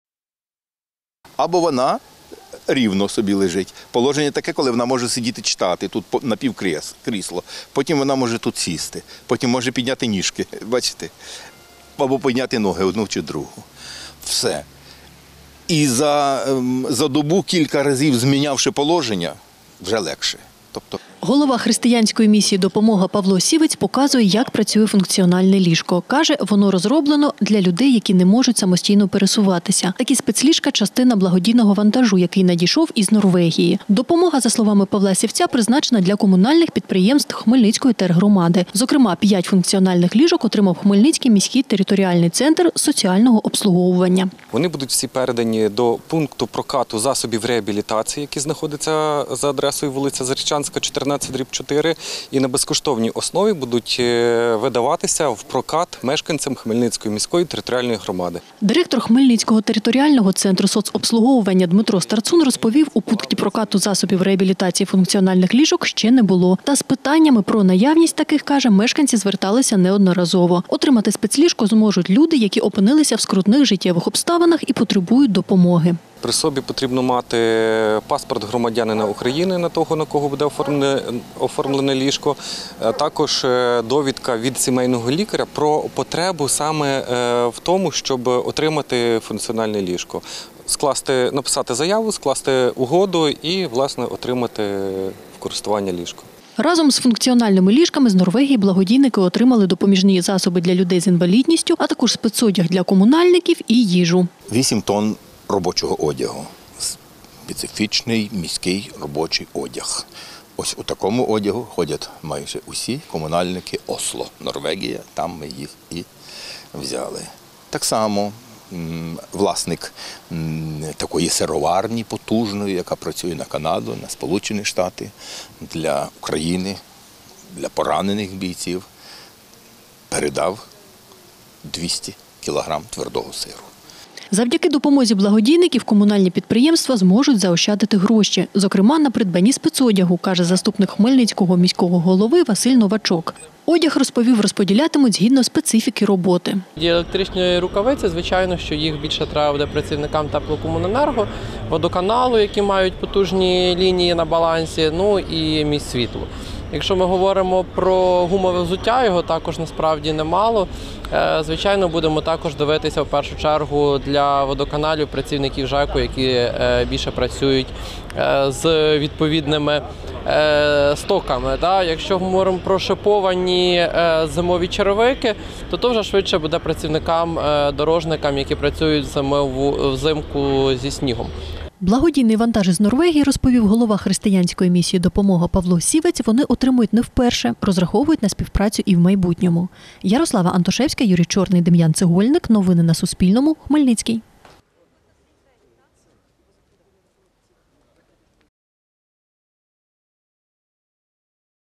Волонтери християнської місії «Допомога» підтримують хмельницьких комунальників
16 тон благодійного вантажу передали волонтери християнської місії «Допомога» Об’єднаної Церкви Християн Віри Євангельської комунальним підприємствам Хмельницької тергромади. Допомога надійшла з Норвегії, розповів голова християнської місії «Допомога» Павло Сівець.